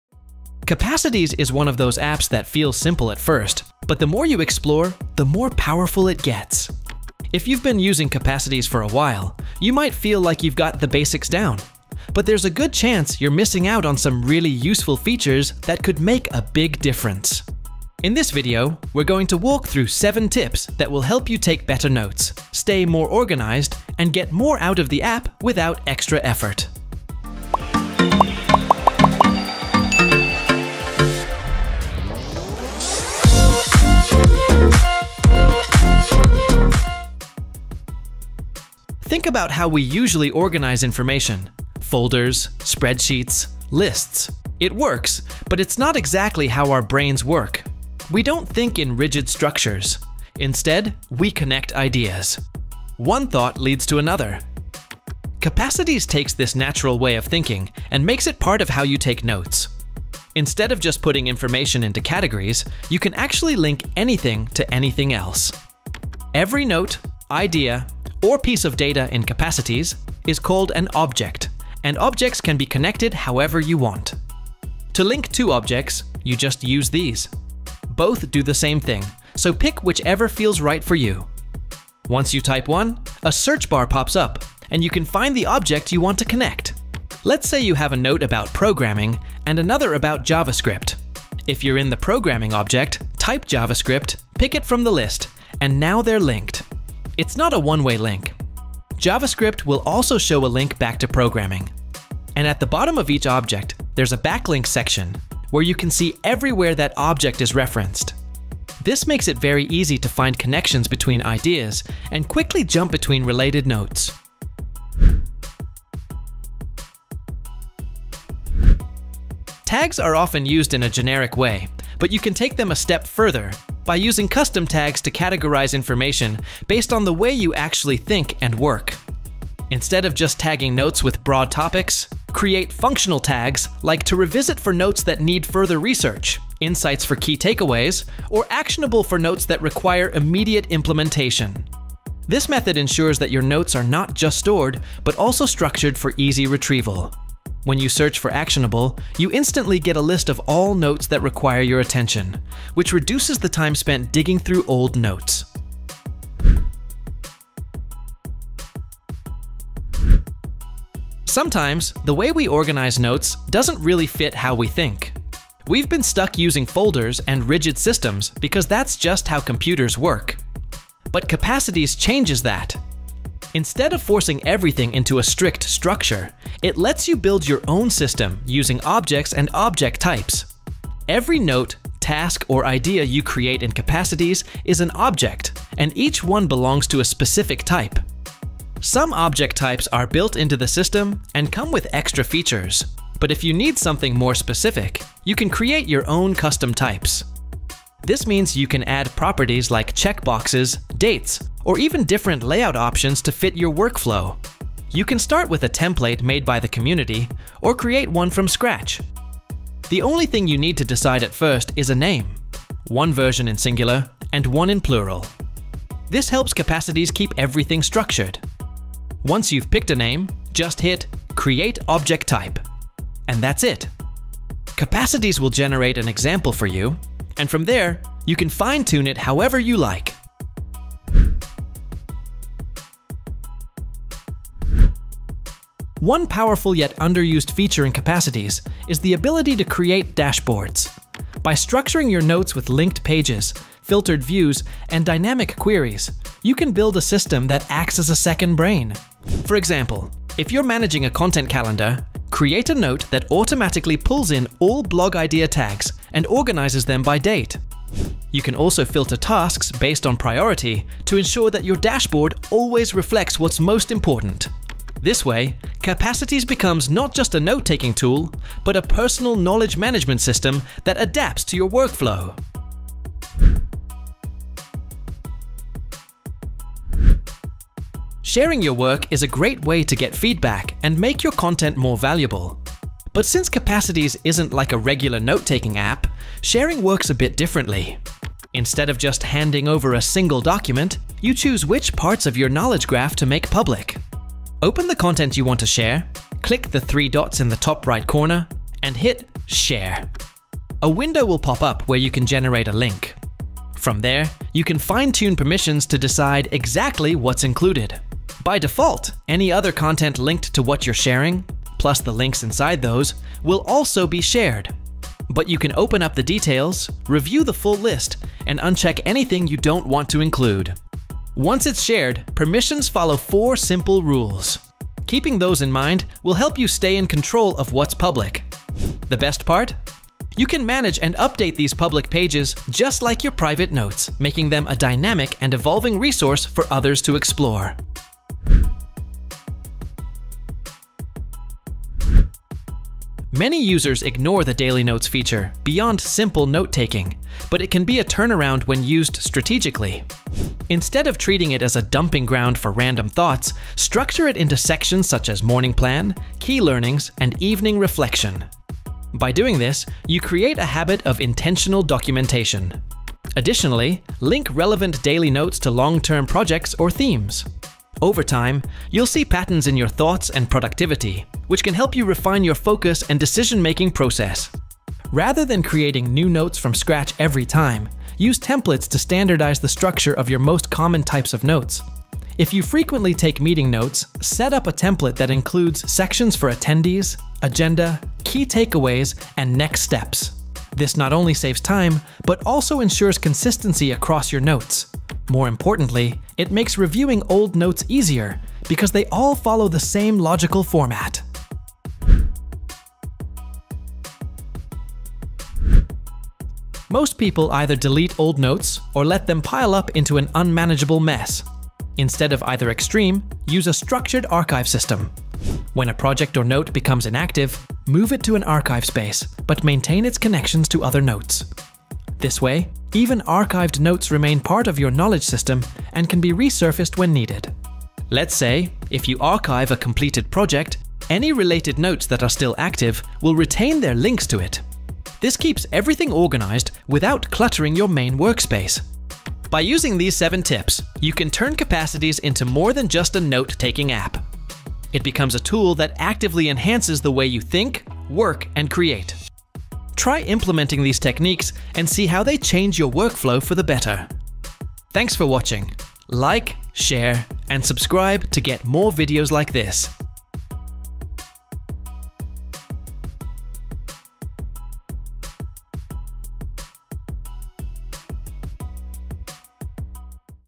7 Useful Capacities Tips & Tricks for Better Note Taking
Capacities is a powerful note-taking application, yet many users only utilize a fraction of its capabilities. If you already have some experience with the app but want to take full advantage of its more advanced features, this guide is for you. We explored seven practical tips that will help you streamline your workflow and enhance the way you organize and interact with your notes.
Intro and Animation Credit:
10Studio: